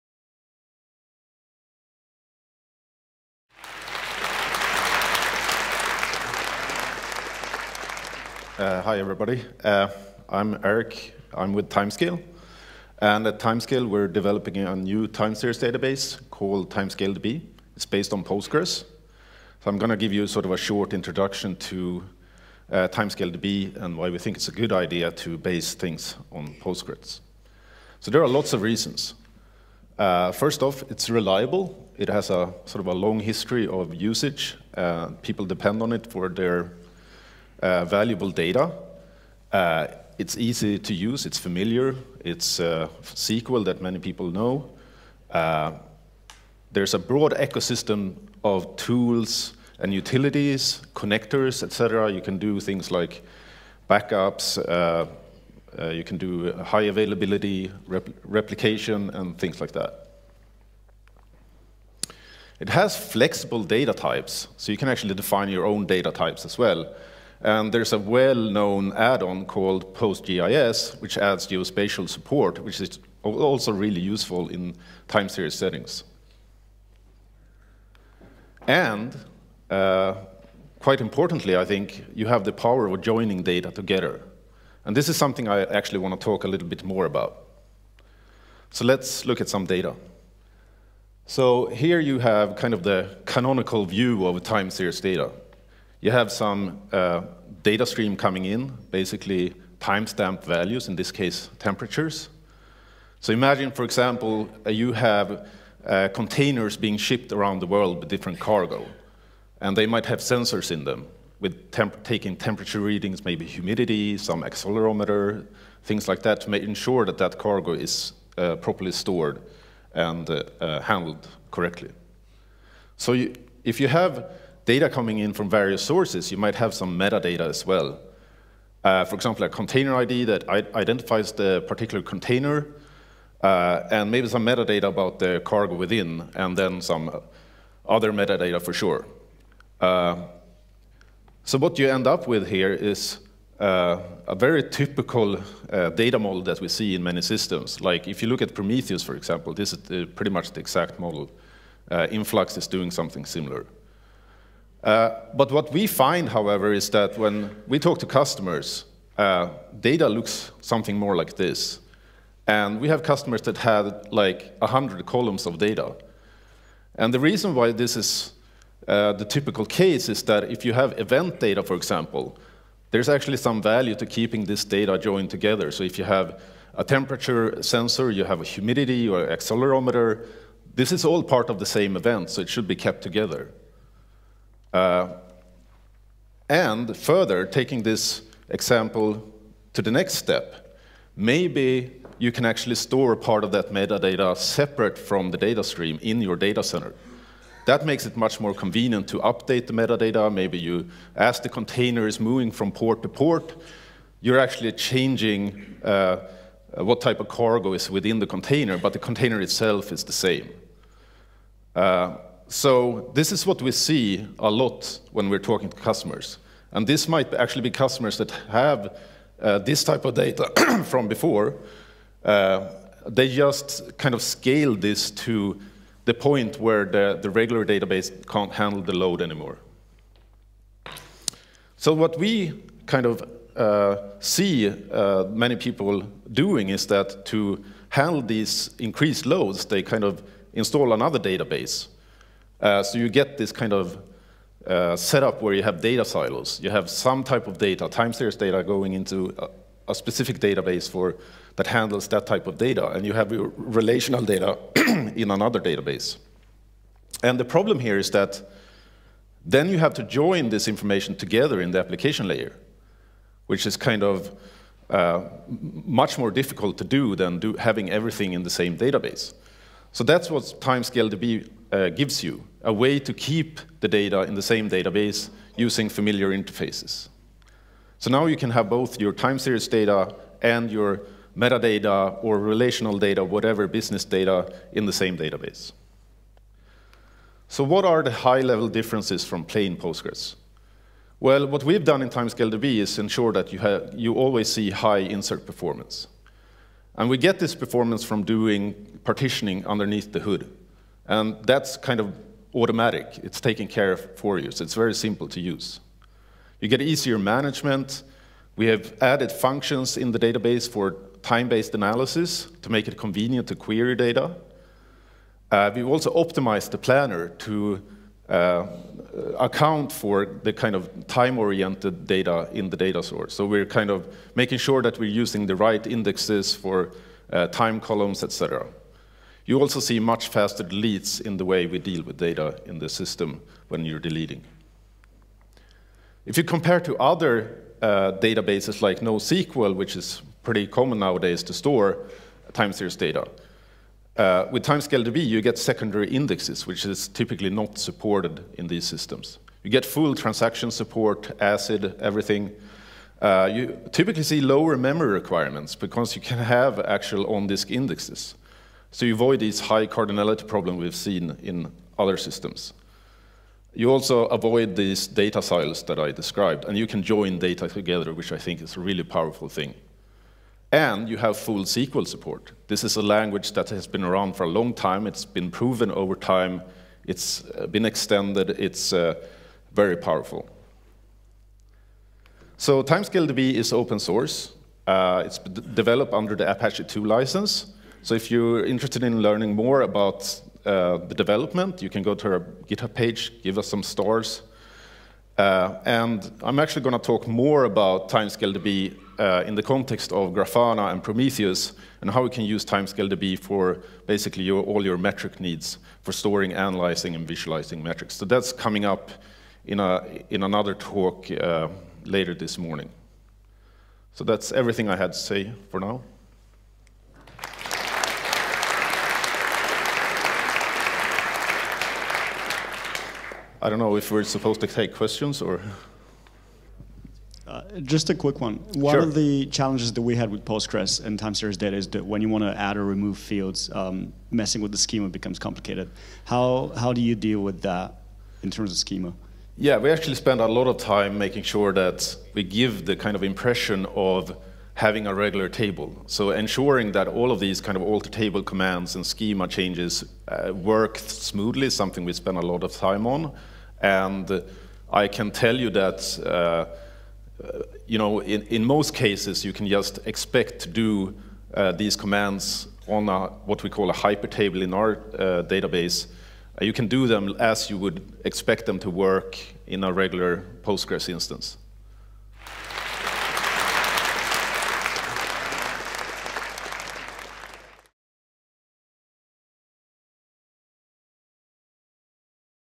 What's New in Timescale
Erik Nordstrom provides an update of TimescaleDB.